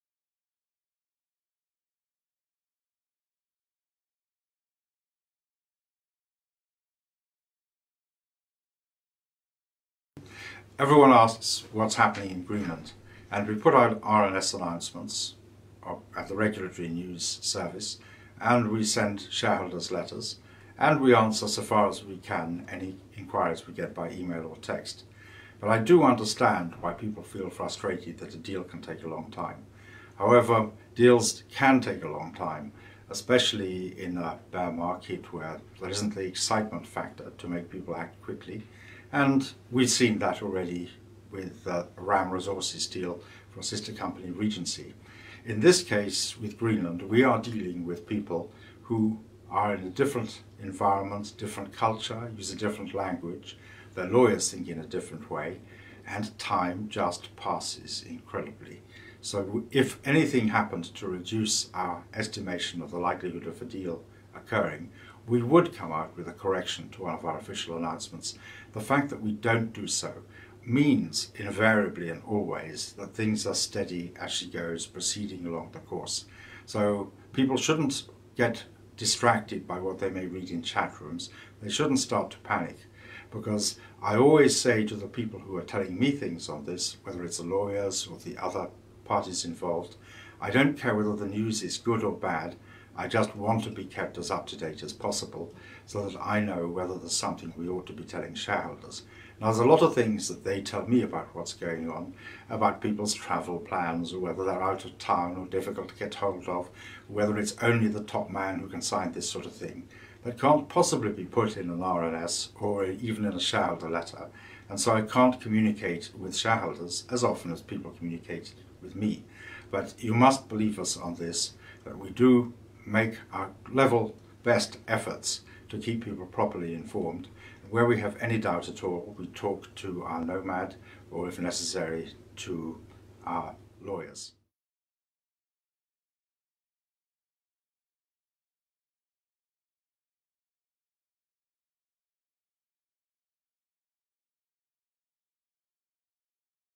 Red Rock Resources Greenland update by Chairman Andrew Bell
Red Rock Resources (AIM: RRR) Chairman Andrew Bell with a brief message regarding news flow and the status of the company's Greenland deal.